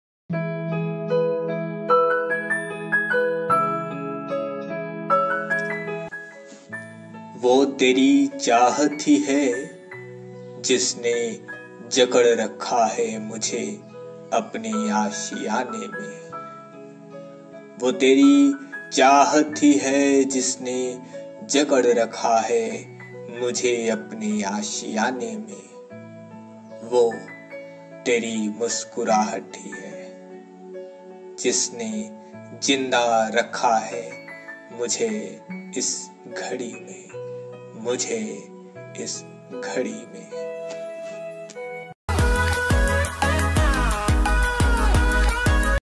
वो तेरी चाहत..| Wo teri chahat | Love Shayari | Yaadein | Corona love | Ishq
This channel provides you poetry, stories, positive thoughts in my voice.

चाहत...
ये शायरी बताती है कि इस महामारी के मुश्किल समय में हमारे प्यार की यादें ही हमें जिन्दा रख रही है । वो प्यार ईश्वर , माता पिता , प्रेमिका आदि से हो सकता है ।

Some more videos link are...